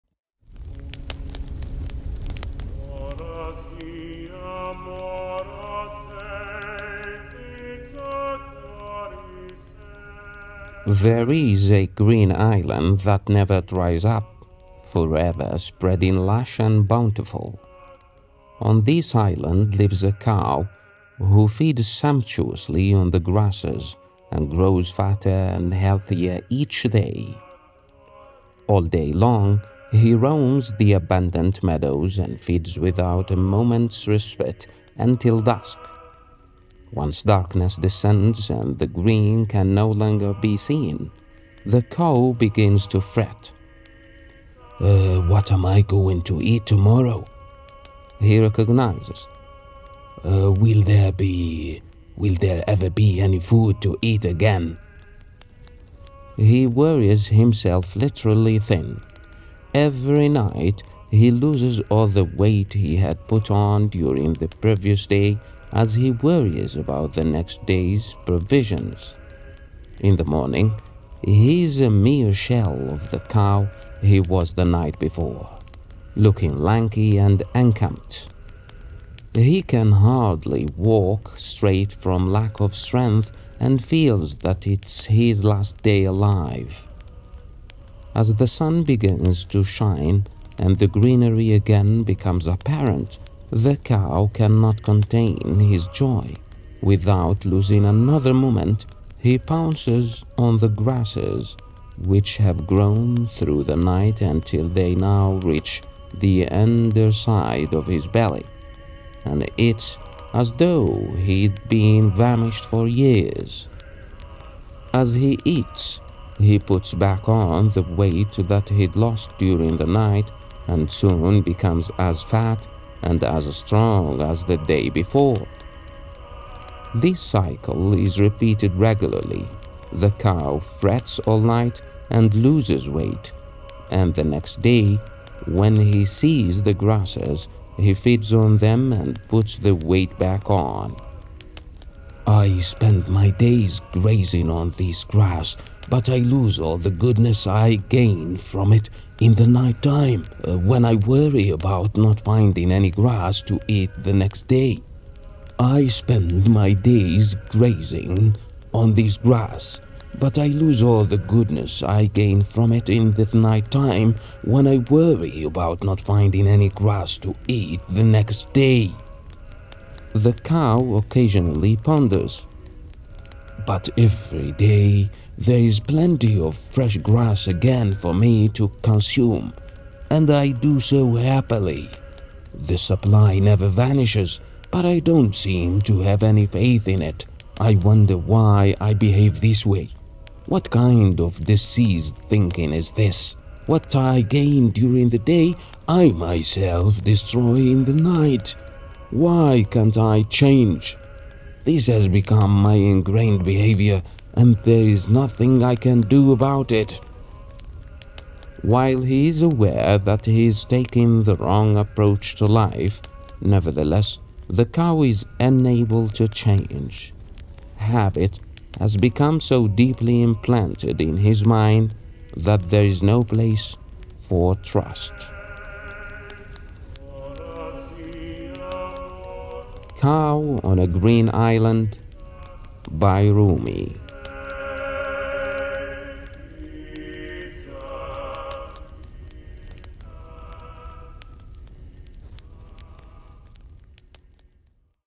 Habit has Become so Deeply Implanted in Mind | Rumi
Cow on a Green Island is a story by Rumi which makes you think about overthinking. Life moves on.